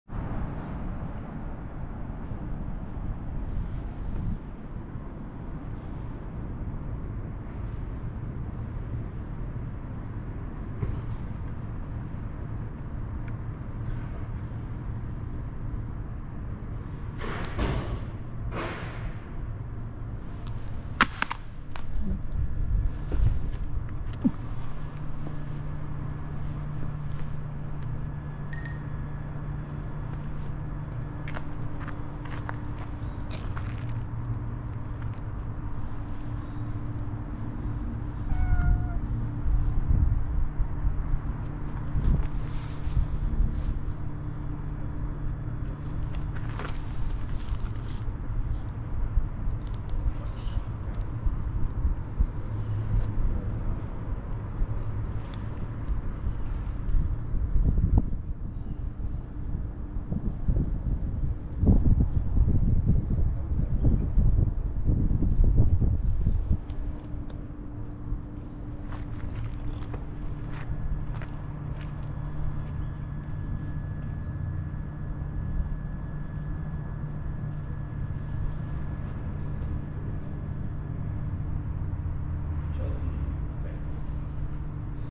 Cat interested in scooter
Curious cat greeted me in Redfern; gave me a chance to test 5.3K resolution 60fps video and to see how youtube handles it! When I uploaded it this morning, there was definitely a full resolution version that said 5K, but having come home from work, I found that that option appeared to have been removed from the resolution selection setting, I don't know why..